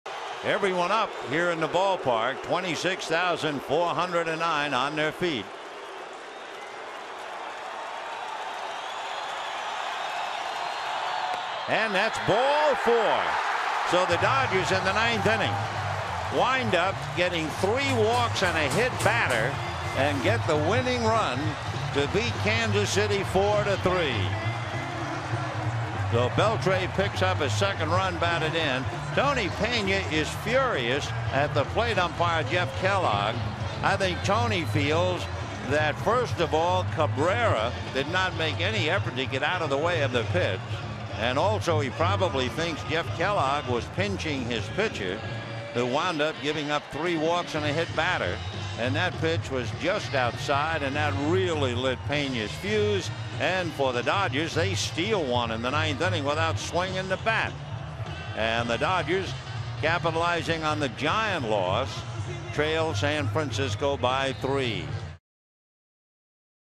Dodgers win on walk to Beltre
6/3/03: The Dodgers win 4-3 after Royals pitcher D.J. Carrasco walks Adrian Beltre